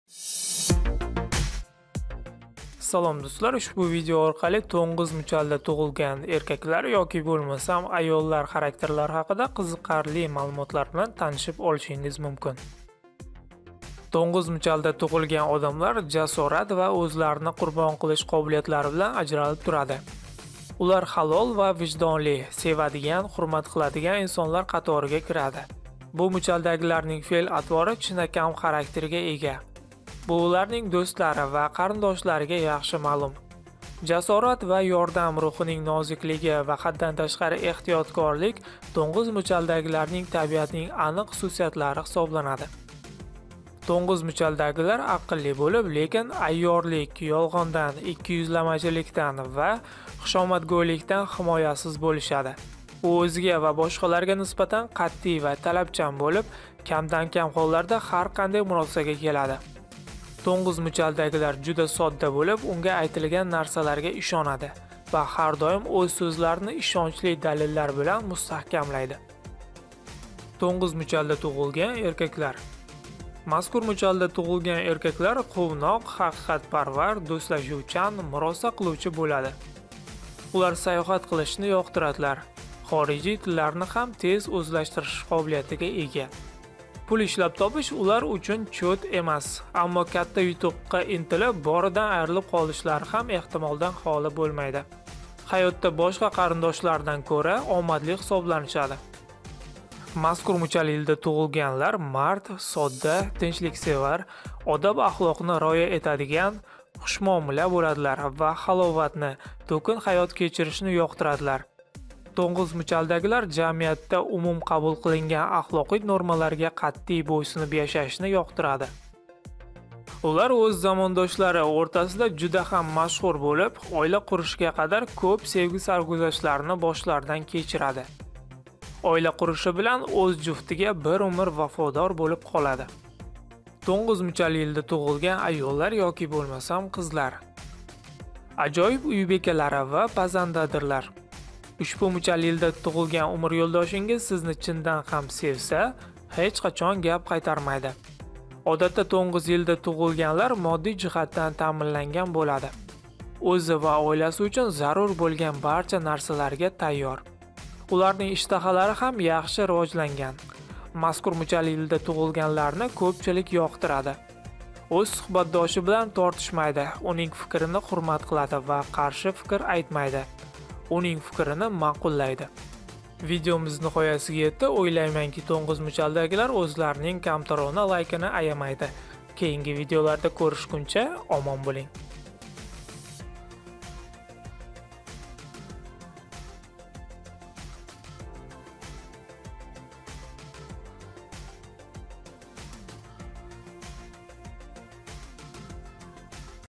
To'ng'iz muchali ERKAK VA AYOLLAR HARAKTERLARI HAQIDA MALUMOT
To’ng’iz muchalida yil muchali tug'ilgan odamlar jasorat va o'zlarini qurbon qilish qobiliyati bilan ajralib turadi. Ular halol va vijdonli, sevadigan, hurmat qiladigan insonlar qatoriga kiradi. Bu muchaldagilarning fe'l-atvori chinakam xarakterga ega, bu ularning do'stlari va qarindoshlariga yaxshi ma'lum. Jasorat va yordam, muchal tili ruhning nozikligi va haddan tashqari ehtiyotkorlik – to’ng’iz muchalidagilarning tabiatining aniq xususiyatlari hisobladi.
To’ngiz muchalidagilar aqlli bo’lib, lekin ayyorlik, yolg'ondan, ikkiyuzlamachilikdan va xushomadgo'ylikdan himoyasiz muchallar.

Muchal haqida

U o'ziga va boshqalarga nisbatan qat'iy va talabchan bo'lib, kamdan-kam hollarda har qanday murosaga keladi. To’ng’iz muchalidagilar juda sodda bo’lib, unga aytilgan narsalarga ishonadi va har doim o'z so'zlarini ishonchli dalillar bilan mustahkamlaydi.